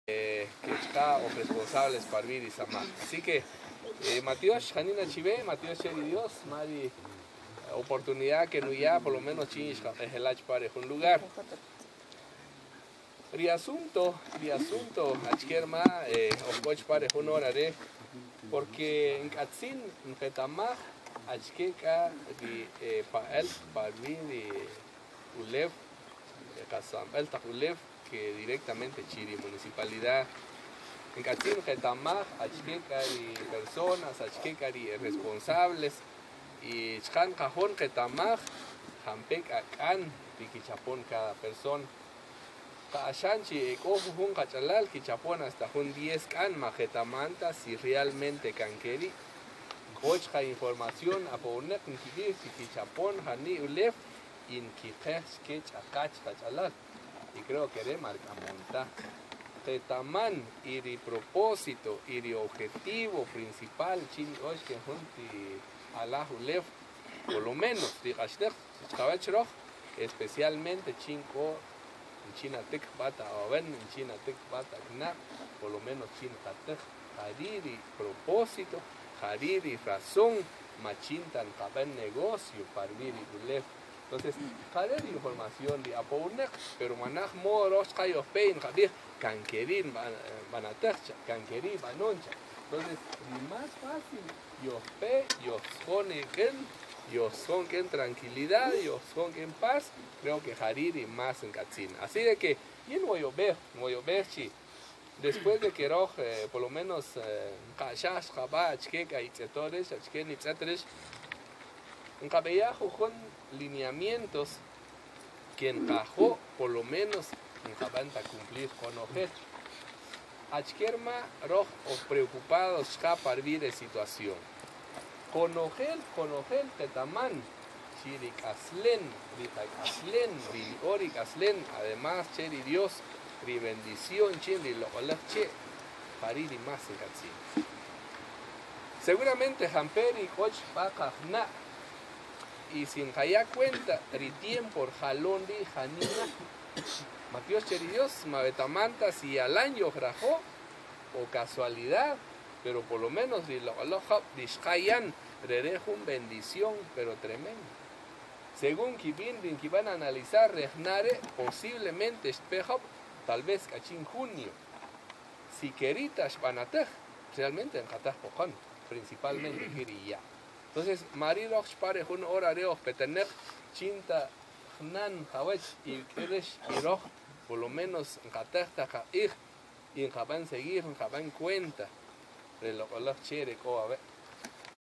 Reunión en astillero municipal con usuarios de terrenos
Patzún, usuarios de terrenos en astillero municipal se reunieron con integrantes del Concejo Municipal y escucharon la preocupación del Alcalde Municipal Reyes Patal Yos.